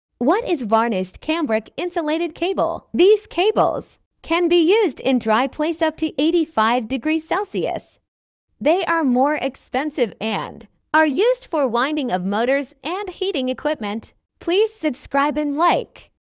what is varnished cambric insulated cable#27#5klike#subscribe
Title: "Unveiling the Excellence of Varnished Cambric Insulated Cables"

Description:
Welcome to our latest YouTube video, where we embark on a captivating journey into the world of Varnished Cambric Insulated Cables. In this enlightening presentation, we explore the intricate details of these remarkable cables, their unique attributes, and the diverse applications that make them a vital component in various industries.

🔌 Key Topics Covered:
1. **Introduction to Varnished Cambric Insulated Cables**: Discover what sets these cables apart in the realm of electrical engineering.

2. **Construction and Materials**: Dive deep into the manufacturing process, understanding the role of varnished cambric and the materials that contribute to the cables' durability.

3. **Advantages**: Explore the benefits of using varnished cambric insulation, including their resistance to temperature variations and electrical performance.

4. **Applications**: Learn about the wide range of industries and sectors that rely on these cables, from industrial machinery to power distribution.

5. **Innovation and Adaptation**: Stay informed about the latest developments in varnished cambric insulated cable technology and how they adapt to evolving needs.

6. **Maintenance and Reliability**: Understand the importance of proper maintenance practices and how these cables offer long-term reliability.

7. **Case Studies**: Witness real-world examples of successful projects powered by varnished cambric insulated cables.

8. **Safety and Sustainability**: Delve into the safety considerations and sustainability aspects associated with these cables.

Join us on this illuminating expedition as we demystify the science and engineering behind Varnished Cambric Insulated Cables. Whether you're a seasoned professional, an aspiring engineer, or simply intrigued by cutting-edge technology, this video is a must-watch!